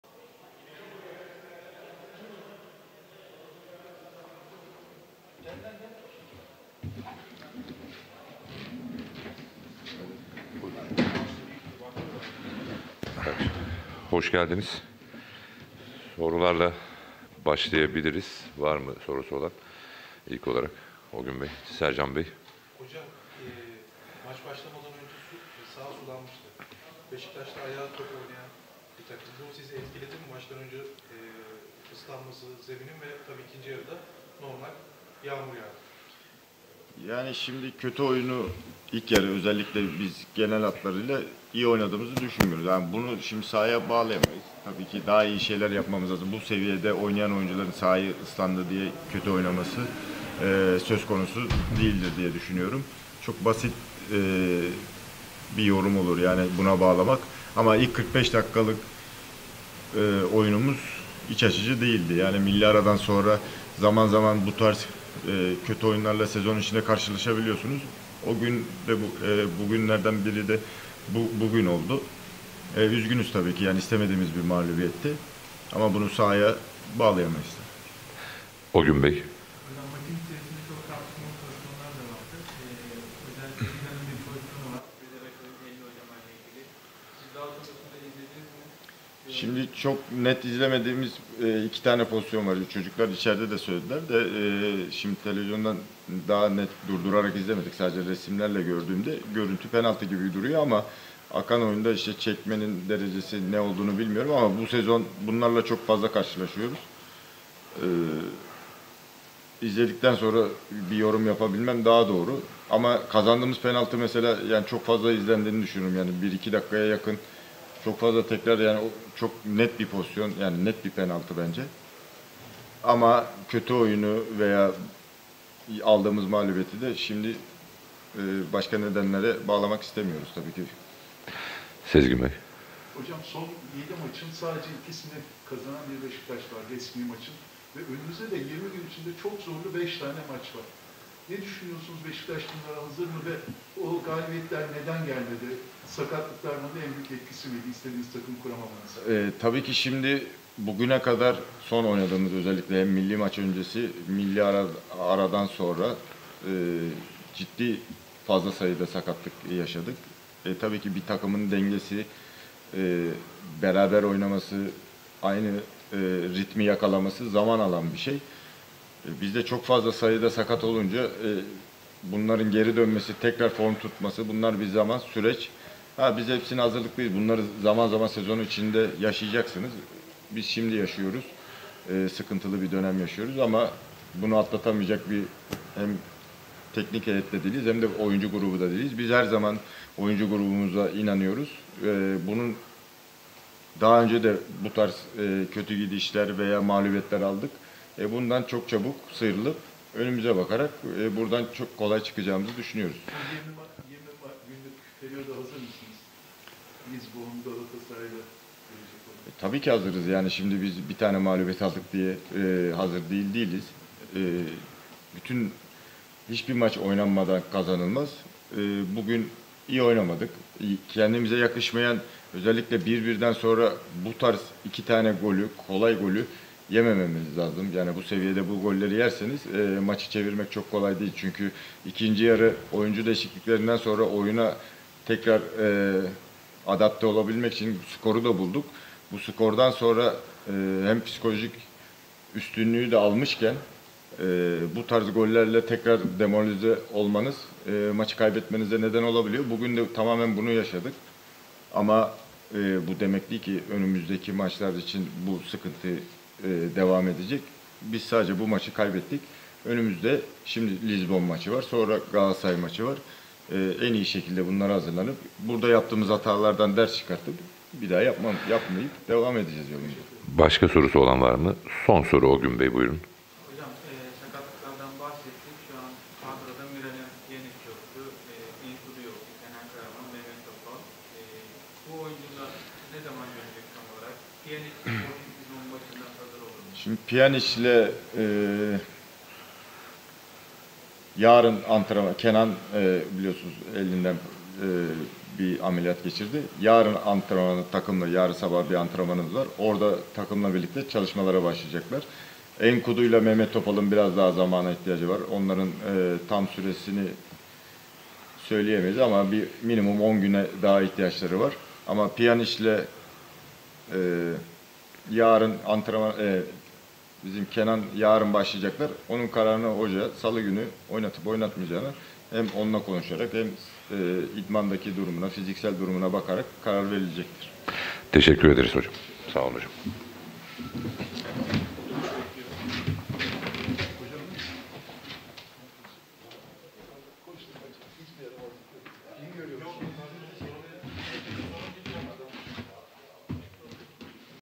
Beşiktaş'ta Murat Şahin, sakatların son durumunu açıkladı! Pjanic, Nkoudou, Kenan Karaman...
Beşiktaş Teknik Sorumlusu Murat Şahin, Başakşehir karşısında istemedikleri bir mağlubiyet aldıklarını ve üzgün olduklarını söyledi.